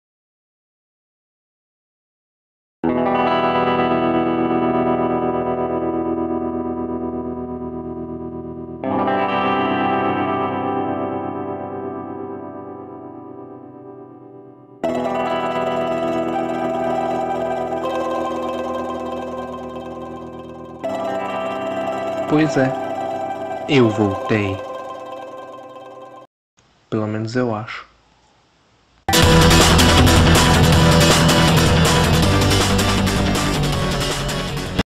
TUDO QUE É RUIM TEM QUE VOLTAR...
Como o próprio título diz, eu voltei...
eu acho.
não tenho certeza.
alguém apoia a minha volta?
porquê é uma volta?
Já tive vários outros canais nessa plataforma.
e nenhum deles deram certo, mas...
AGORA VAI...EU ESPERO...

Isso serve de lição pra mostrar que tem coisas piores que a quarentena, e uma delas é eu voltar a fazer vídeos.